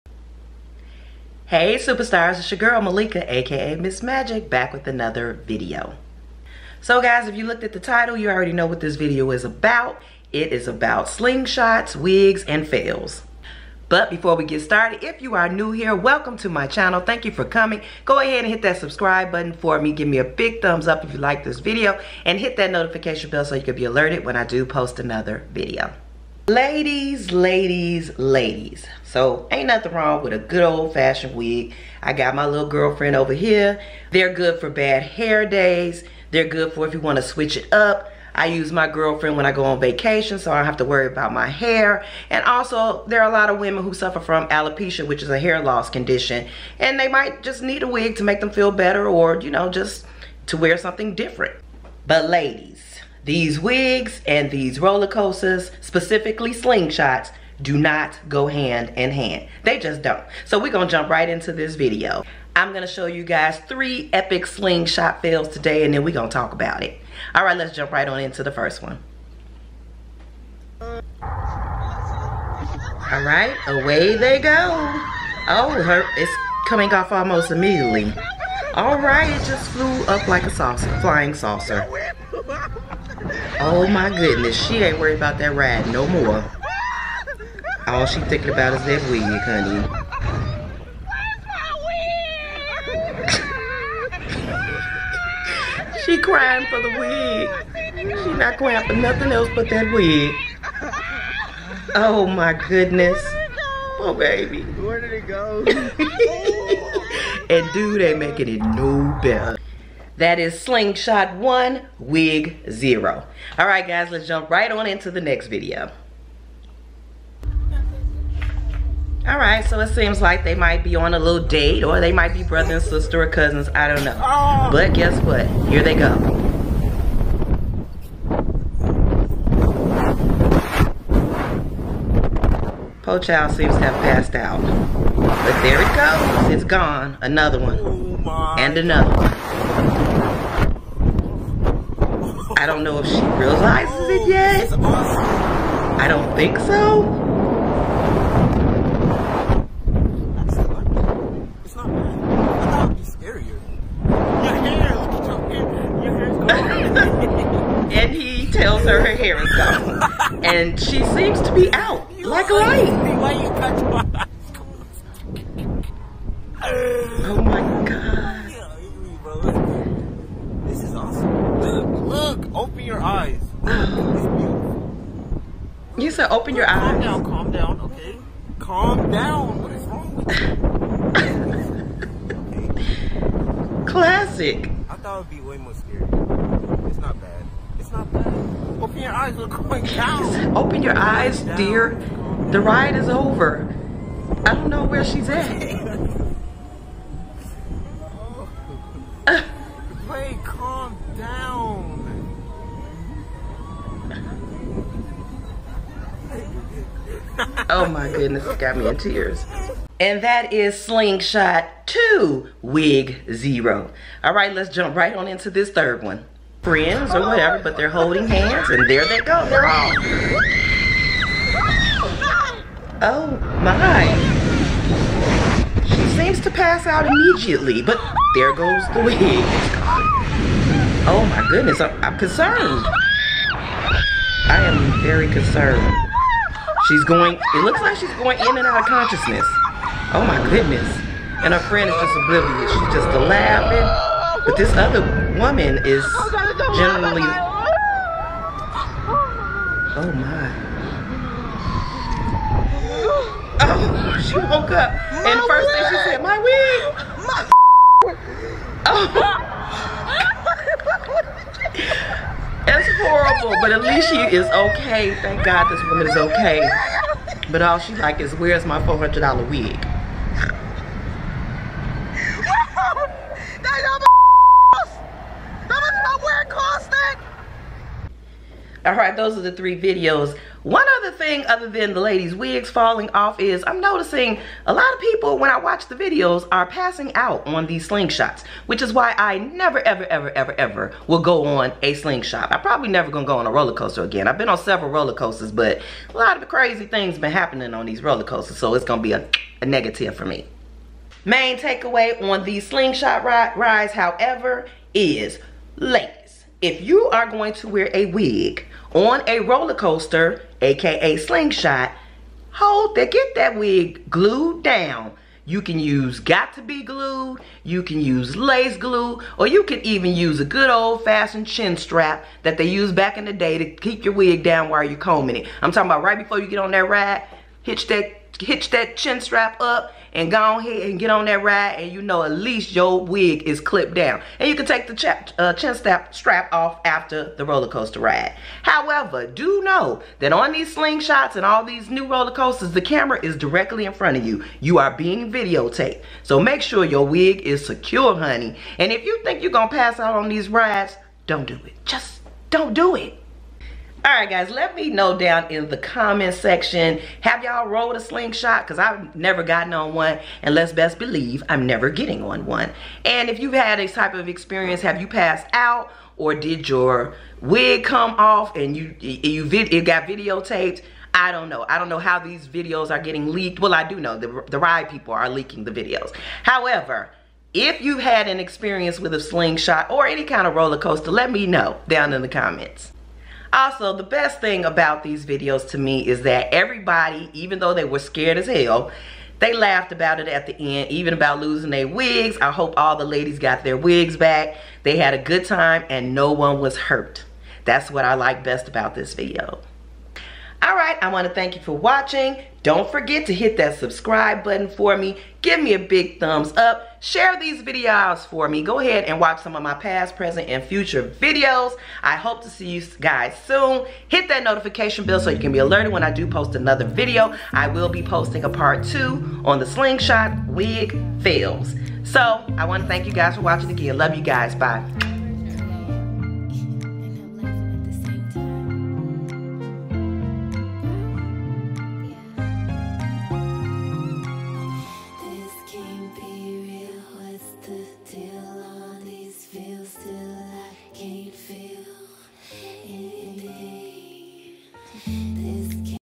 Hairstylist reacts To SlingShot Ride Wig Snatch 😳
Wear a wig on a SlingShot at your own risk